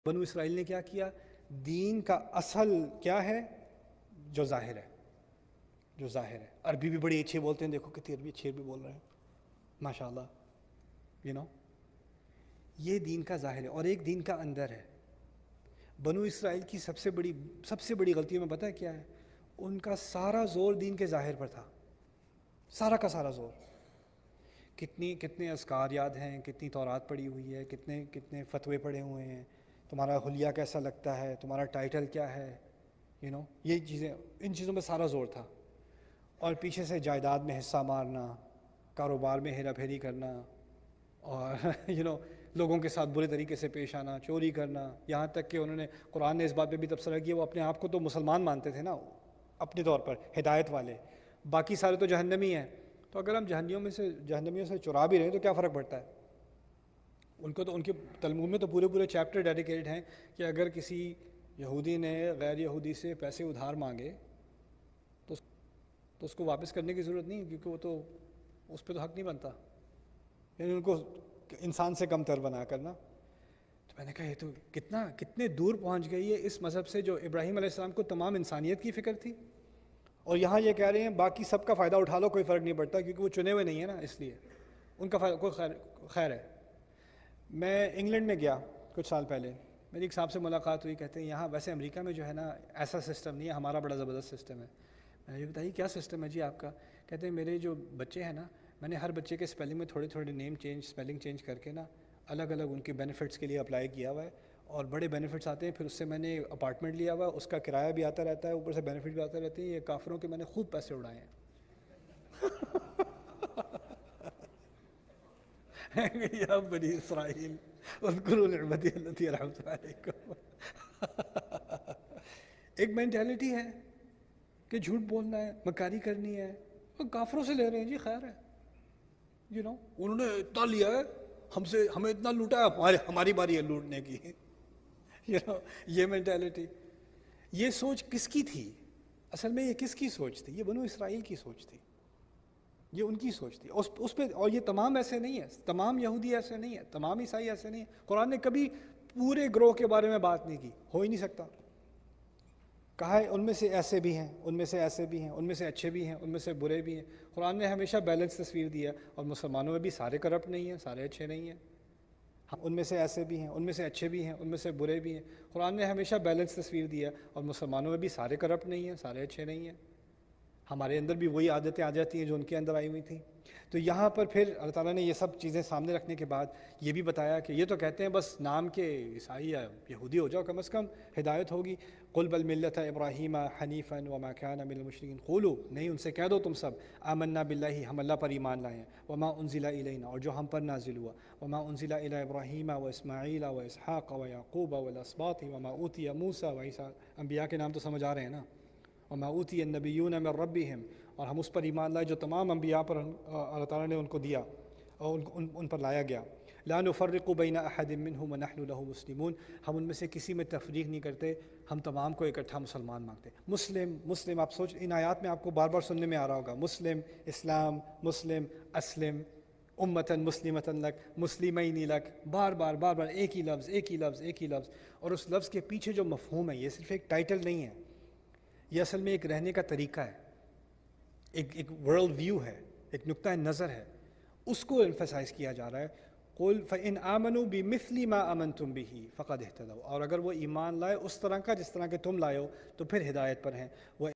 Banu Israel Ki Galti | Nouman Ali Khan | Urdu
Nouman Ali Khan talks about the mistakes of the people of Israel.